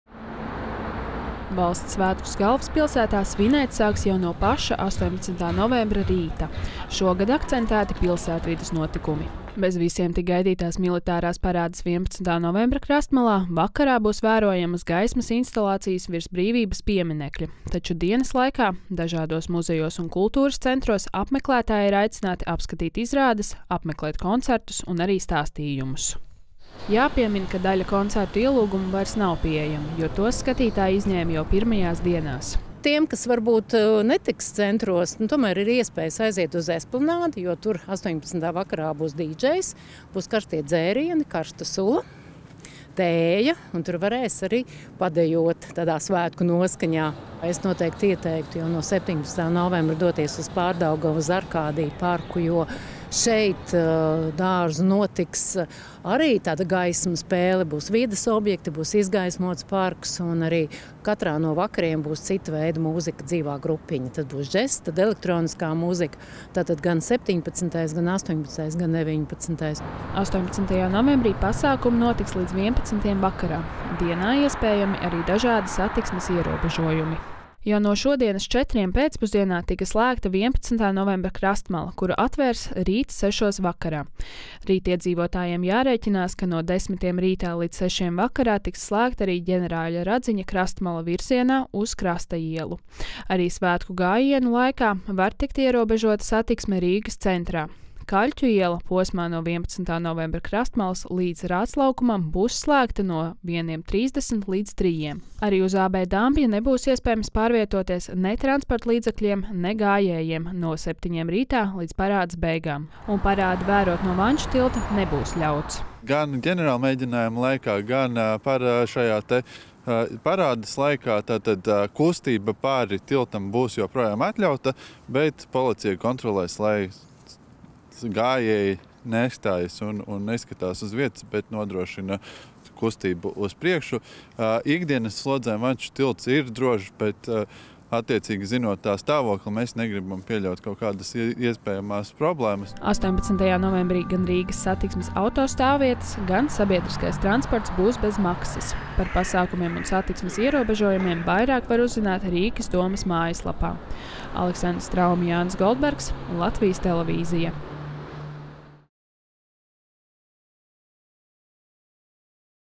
Rīgā 18. novembrī – dažādi pasākumi un satiksmes ierobežojumi
Rīt, Valsts svētku dienā, Rīgā visas dienas garumā paredzēti dažādi pasākumi. Sākot ar gaismas instalācijām, beidzot ar koncertiem. Notiks arī gadskārtējā Nacionālo bruņoto spēku parāde, un tās laikā ir paredzēti vairāki satiksmes ierobežojumi.